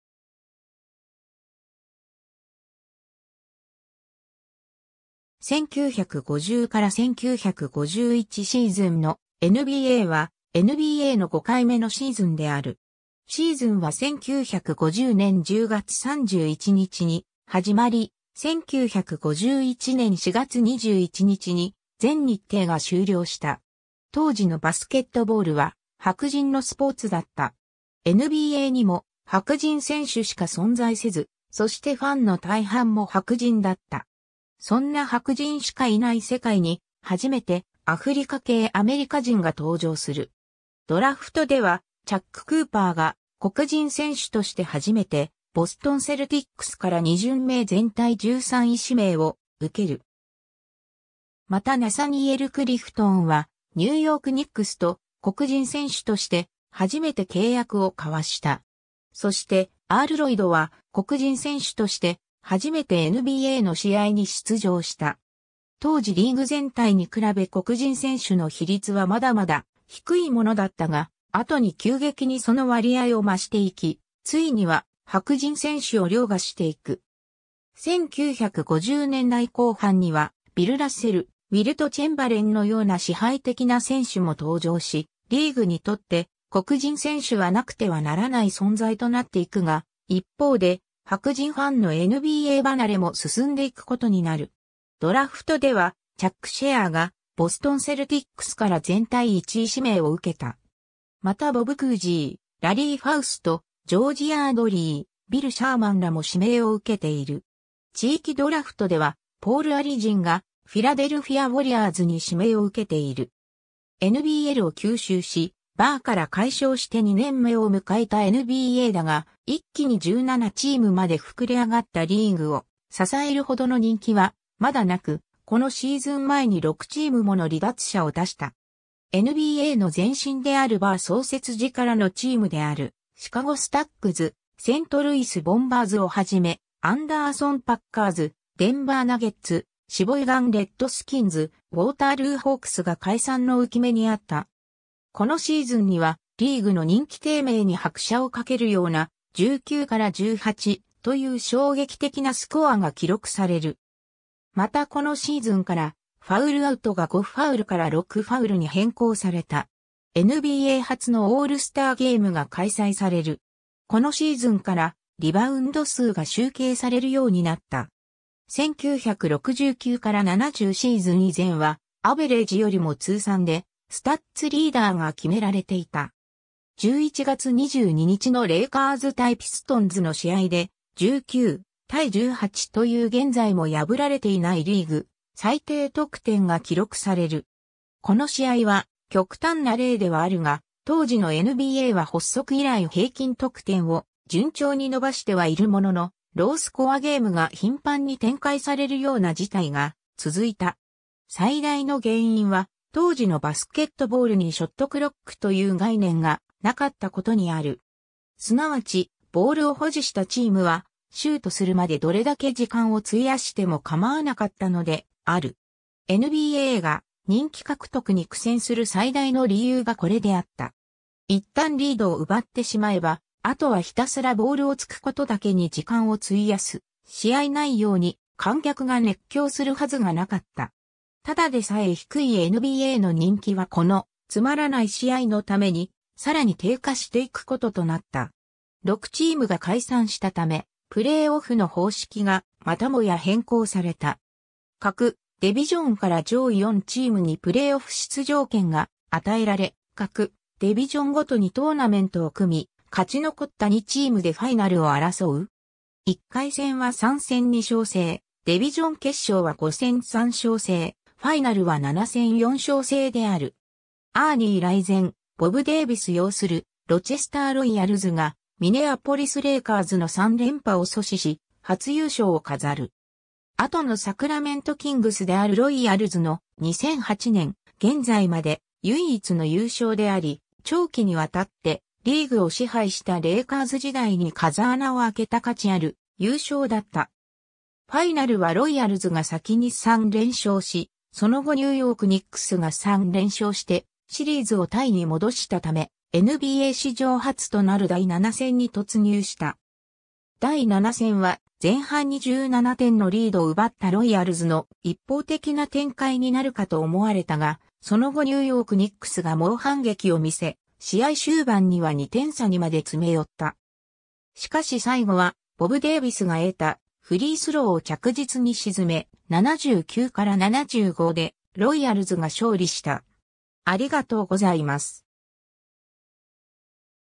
1950-1951シーズンのNBA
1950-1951シーズンのNBAは、NBAの5回目のシーズンである。
シーズンは1950年10月31日に始まり、1951年4月21日に全日程が終了した。
 当時のバスケットボールは白人のスポーツだった。
NBAにも白人選手しか存在せず、そしてファンの大半も白人だった。
そんな白人しか居ない世界に、初めてアフリカ系アメリカ人が登場する。
ドラフトではチャック・クーパーが、黒人選手として初めてボストン・セルティックスから2巡目全体13位指名を受ける。
またナサニエル・クリフトンはニューヨーク・ニックスと、黒人選手として初めて契約を交わした。
そしてアール・ロイドは黒人選手として初めてNBAの試合に出場した。
 当時リーグ全体に比べ黒人選手の比率はまだまだ低いものだったが、後に急激にその割合を増していき、ついには白人選手を陵駕していく。
1950年代後半にはビル・ラッセル、ウィルト・チェンバレンのような支配的な選手も登場し、リーグにとって黒人選手はなくてはならない存在となっていくが、一方で白人ファンのNBA離れも進んでいくことになる。
 ドラフトではチャック・シェアーがボストン・セルティックスから全体1位指名を受けた。
またボブ・クージー、ラリー・ファウスト、ジョージ・ヤードリー、ビル・シャーマンらも指名を受けている。
地域ドラフトではポール・アリジンがフィラデルフィア・ウォリアーズに指名を受けている。
 NBLを吸収し、BAAから改称して2年目を迎えたNBAだが、一気に17チームまで膨れ上がったリーグを支えるほどの人気はまだなく、このシーズン前に6チームもの離脱者を出した。
NBAの前身であるBAA創設時からのチームであるシカゴ・スタッグズ、セントルイス・ボンバーズを始め、アンダーソン・パッカーズ、デンバー・ナゲッツ、シボイガン・レッドスキンズ、ウォータールー・ホークスが解散の憂き目に遭った。
このシーズンにはリーグの人気低迷に拍車を掛けるような、19-18という衝撃的なスコアが記録される。
 またこのシーズンからファウルアウトが5ファウルから6ファウルに変更された。
 NBA初のオールスターゲームが開催される。
 ※このシーズンからリバウンド数が集計されるようになった。
1969-70シーズン以前はアベレージよりも通算でスタッツリーダーが決められていた。
 11月22日のレイカーズ対ピストンズの試合で、19対18という現在も破られていないリーグ最低得点が記録される。
 この試合は極端な例ではあるが、当時のNBAは発足以来平均得点を順調に伸ばしてはいるものの、ロースコアゲームが頻繁に展開されるような事態が続いた。
最大の原因は当時のバスケットボールにショットクロックという概念がなかったことにある。
すなわち、ボールを保持したチームはシュートするまでどれだけ時間を費やしても構わなかったのである。
 NBAが人気獲得に苦戦する最大の理由がこれであった。
一旦リードを奪ってしまえば、後はひたすらボールを突くことだけに時間を費やす試合内容に、観客が熱狂するはずがなかった。
ただでさえ低いNBAの人気はこの「つまらない試合」のためにさらに低下していくこととなった。
 6チームが解散したため、プレーオフの方式がまたもや変更された。
各デビジョンから上位4チームにプレーオフ出場権が与えられ、各デビジョンごとにトーナメントを組み、勝ち残った2チームでファイナルを争う。
1回戦は3戦2勝制、デビジョン決勝は5戦3勝制、ファイナルは7戦4勝制である。
 アーニー・ライゼン、ボブ・デイヴィス擁するロチェスター・ロイヤルズがミネアポリス・レイカーズの三連覇を阻止し、初優勝を飾る。
後のサクラメント・キングスであるロイヤルズの、2008年現在まで唯一の優勝であり、長期に渡ってリーグを支配したレイカーズ時代に風穴を開けた価値ある優勝だった。
ファイナルはロイヤルズが先に3連勝し、その後ニューヨーク・ニックスが3連勝してシリーズをタイに戻したため、NBA史上初となる第7戦に突入した。
第7戦は前半に17点のリード奪ったロイヤルズの一方的な展開になるかと思われたが、その後ニューヨーク・ニックスが猛反撃を見せ、試合終盤には2点差にまで詰め寄った。
しかし最後はボブ・デイヴィスが得たフリースローを着実に沈め、79-75でロイヤルズが勝利した。